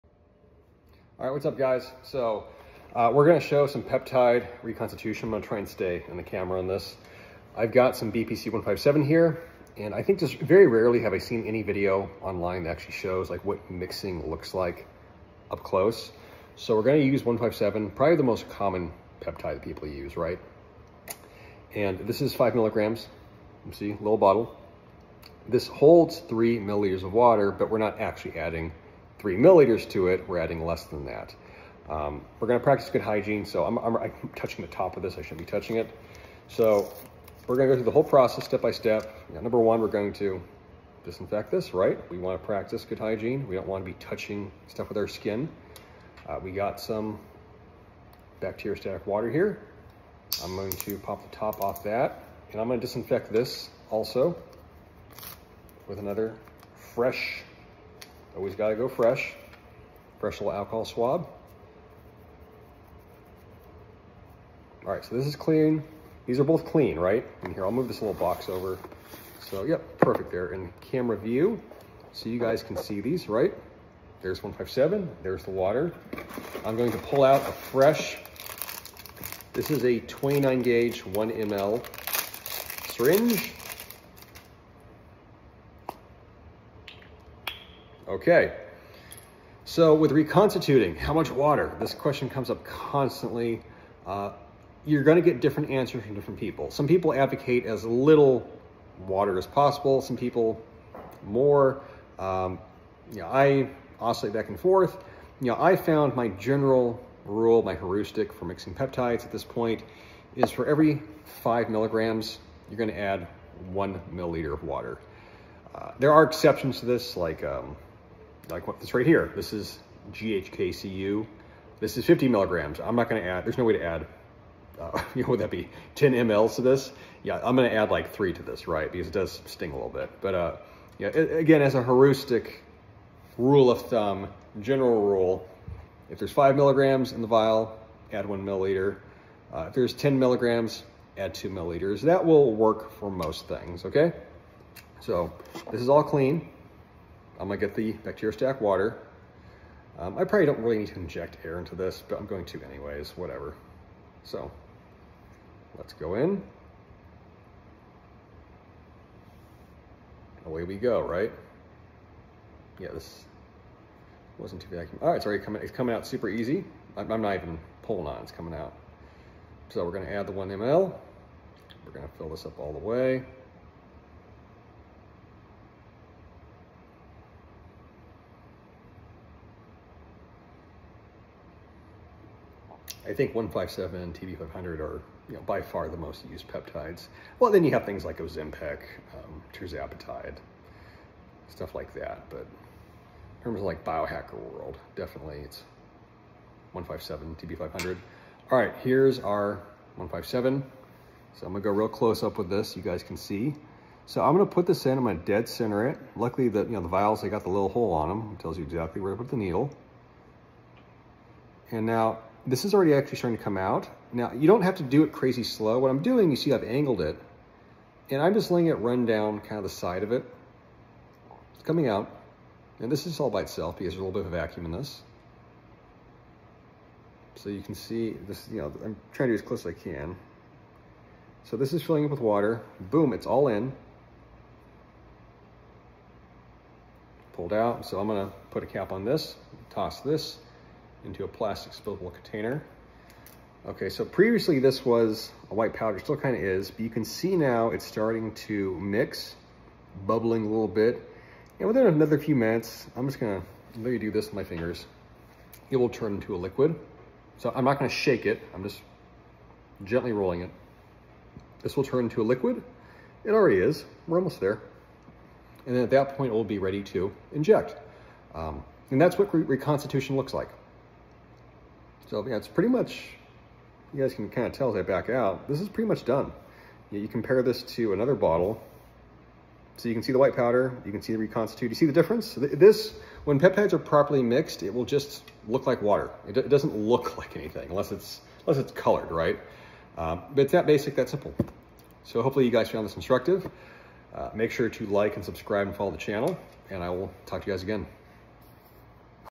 How to Reconstitute Peptides
This simple tutorial shows how to add bacteriostatic water to your peptides and properly reconstitute them